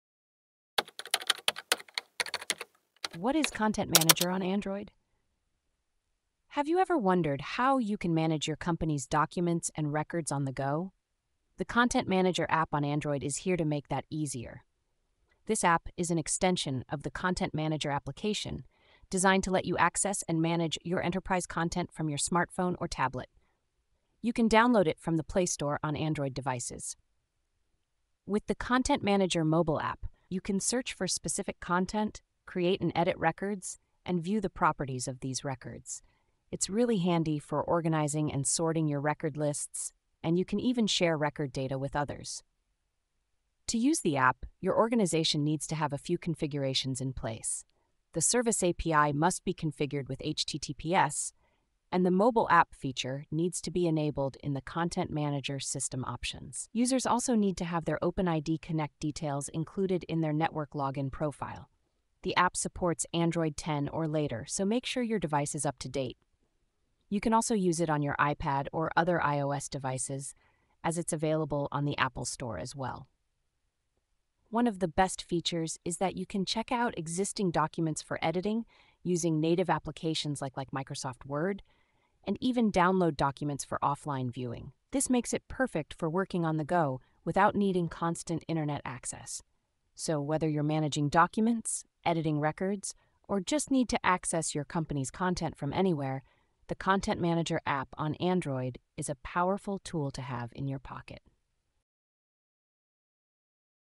What Is Content Manager On Android? - Be App Savvy
What Is Content Manager On Android? In this informative video, we'll take a closer look at the Content Manager app on Android and how it can streamline your document management tasks. This app is designed specifically for accessing and managing enterprise content directly from your smartphone or tablet, making it an essential tool for professionals on the move. We will discuss the key features that the app offers, including the ability to search for specific content, create and edit records, and view record properties.

Additionally, we will guide you through the necessary configurations required by your organization to use the app effectively. You’ll learn about the compatibility requirements, ensuring that your device is ready to support the app’s functionalities. We will also highlight how the app allows for document editing using familiar applications like Microsoft Word and the convenience of offline access to documents.

Whether you're a business professional looking to manage your documents efficiently or someone who needs quick access to your company's records, the Content Manager app is a practical solution. Join us in this video to discover how this app can simplify your workflow and keep you connected to your essential documents anytime, anywhere.